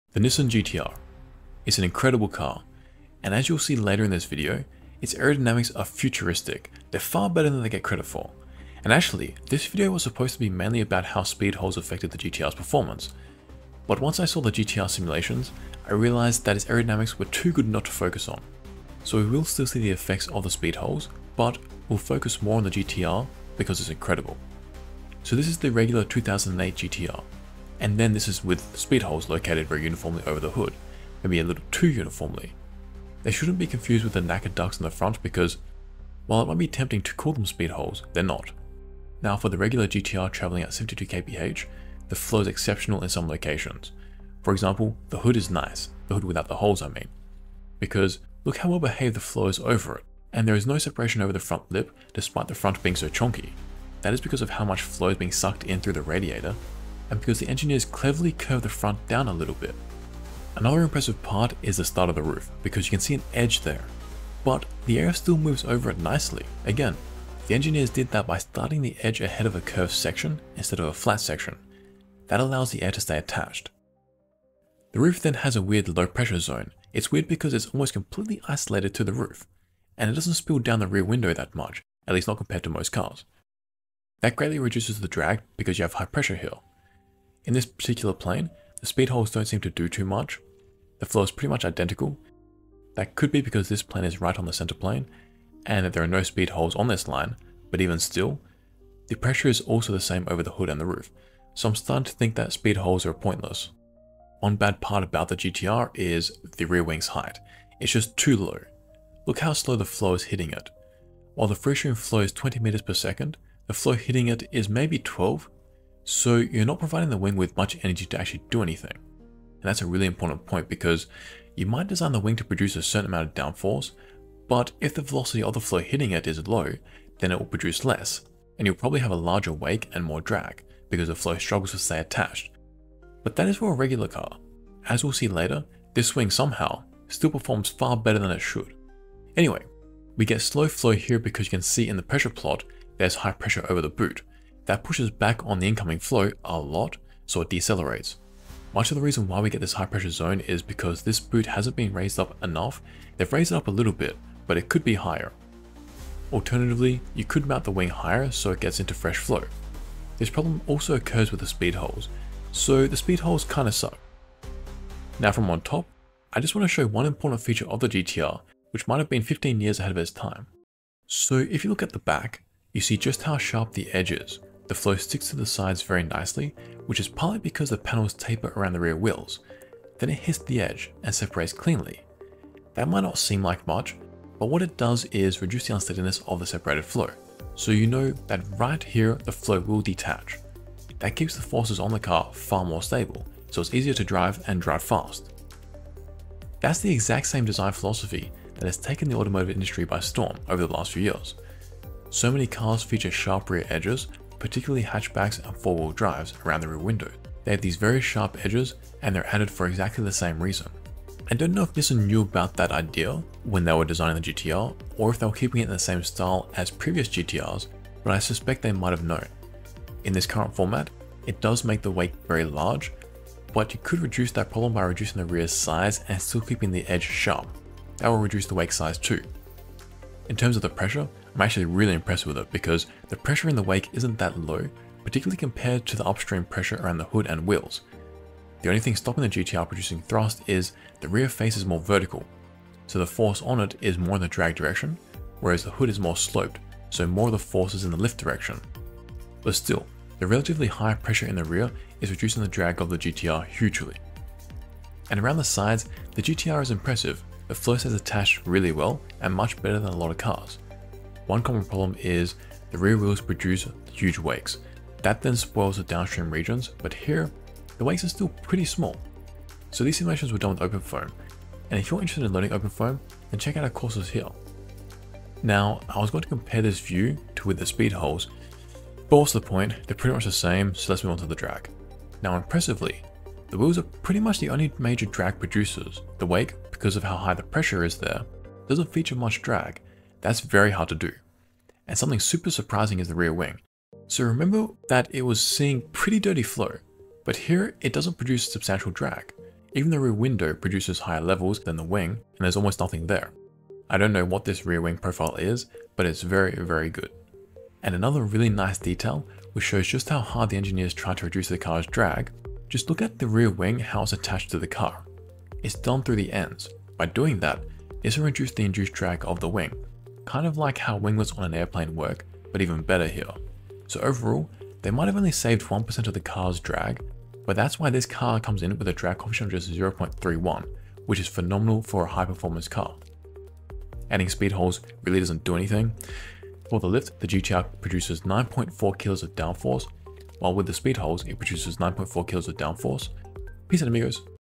Nissan GTR’s Futuristic Aerodynamics
Details about simulation: MRFs for the wheels, model is as accurate as we can get. Meant for general entertainment and general aero.